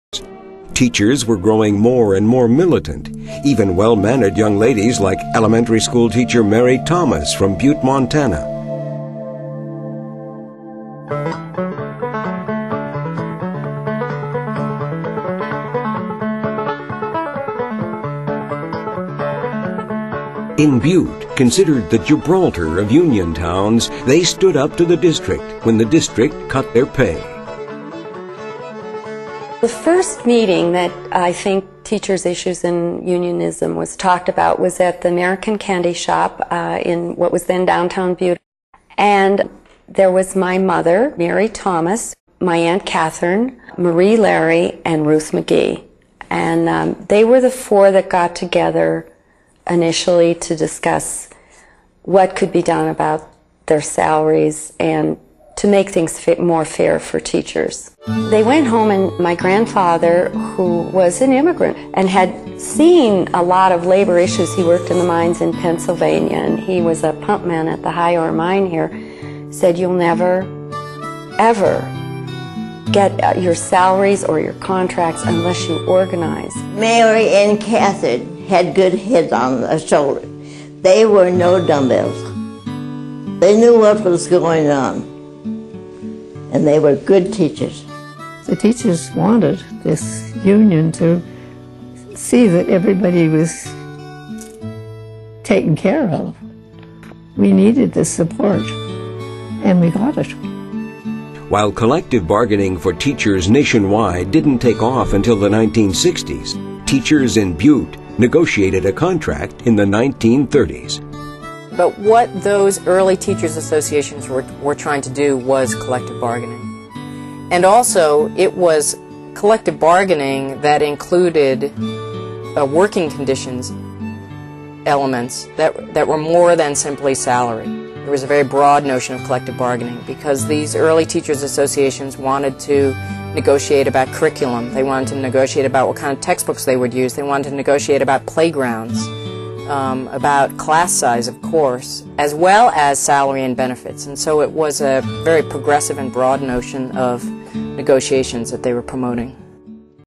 AFT History Video: Teachers Organize in Butte, MT
Teachers Organize in Butte, MT.  Chapter 4 in the AFT History video.